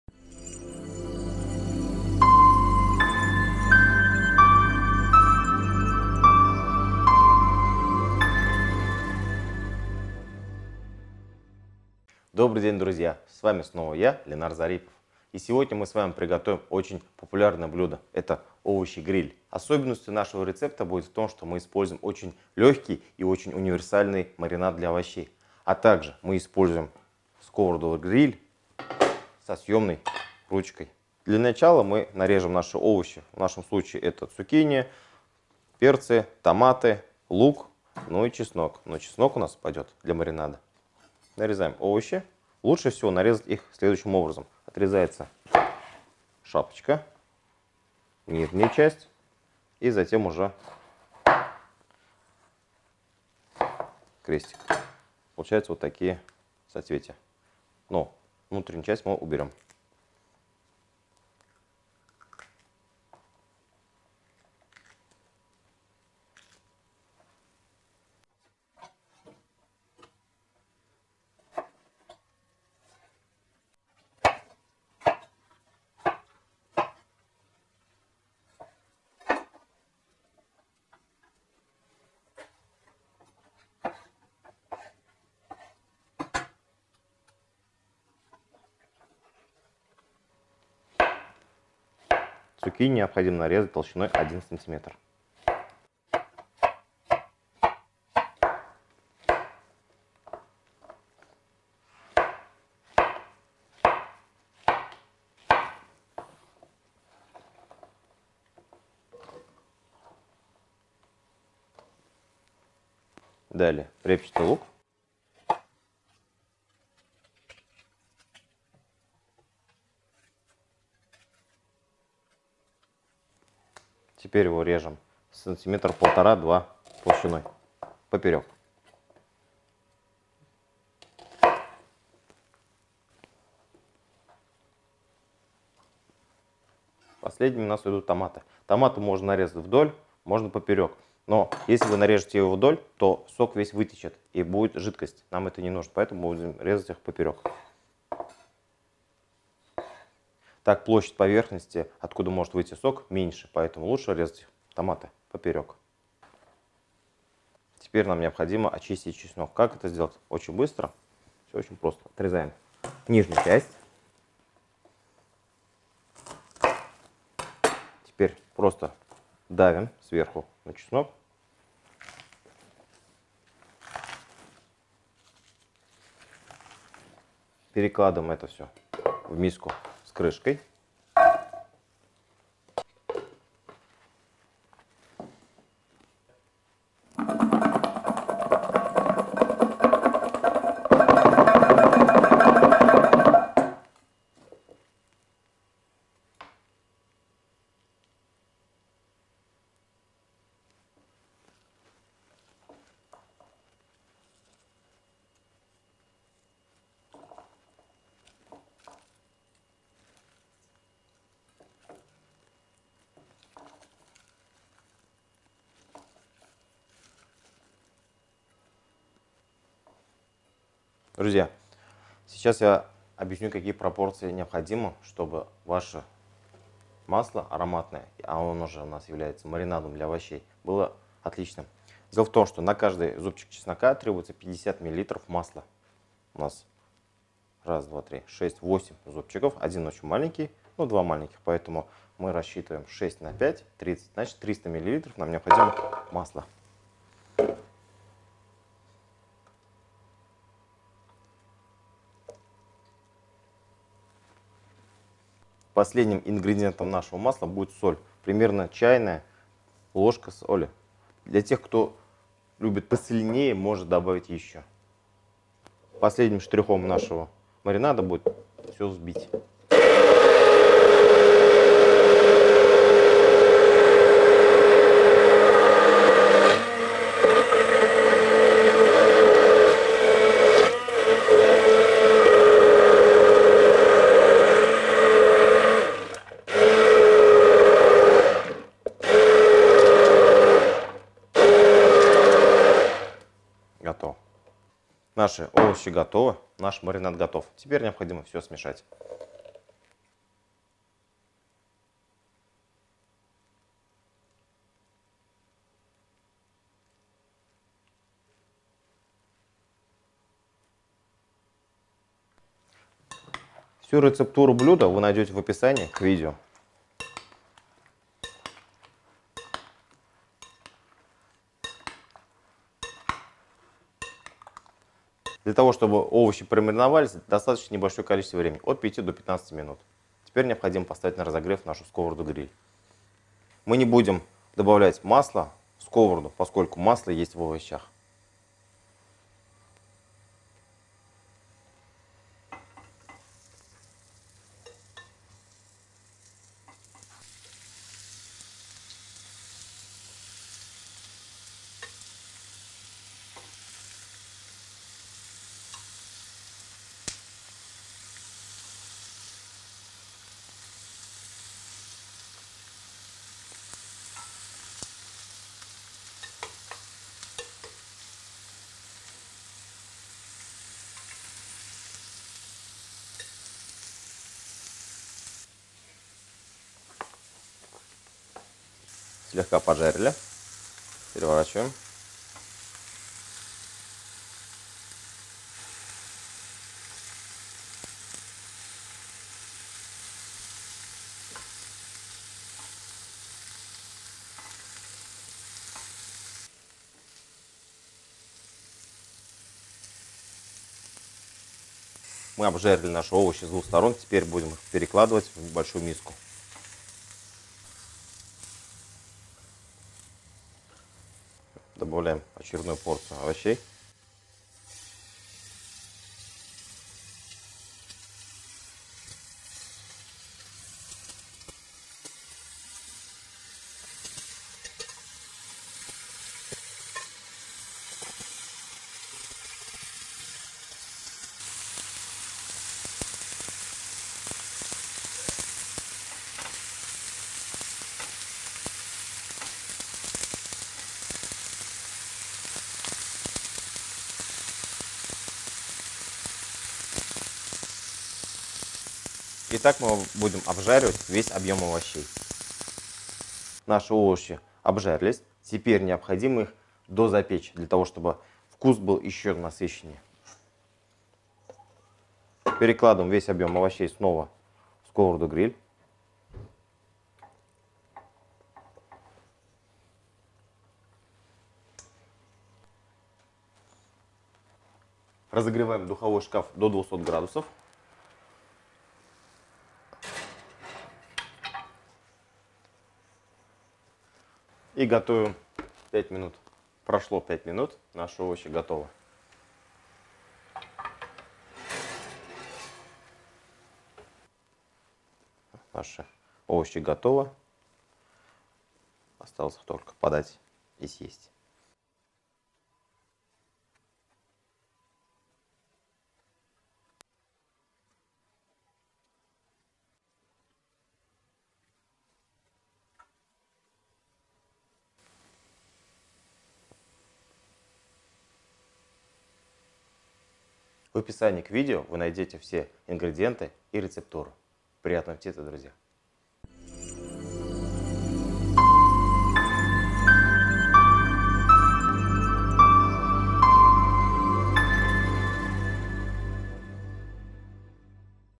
Овощи гриль запеченные в духовке [а так же простой рецепт маринада]
Для приготовления блюда нам понадобится сковорода гриль с высоким бортом и съемной ручкой Мечта™, которую можно приобрести

Первое, с чего необходимо начать готовить овощи гриль, это приготовить маринад для овощей. Для этого необходимо налить в чашу блендера растительное масло, добавить чеснок, соль и все измельчить до однородной консистенции.
Далее, необходимо нарезать овощи и сложить их в миску, сверху залить маринадом и оставить мариноваться в холодильник. Маринованные овощи необходимо жарить на сильно разогретой сковороде гриль с высоким бортом и съемной ручкой. Лучше всего жарить небольшими партиями и складывать овощи в миску.
Как только весь объем овощей поджарится, необходимо все это переложить снова в сковороду и запечь в духовом шкафу при температуре 200 градусов 7 минут. По истечению времени вынимаем из духового шкафа и перекладываем овощи на блюдо.
Приятного аппетита!

Ингредиенты:
2 шт болгарский перец,
5 шт., томаты,
2 шт репчатый лук,
2 шт. цукини, соль и перец по вкусу,
300 мл растительное масло,
6 зубчиков чеснока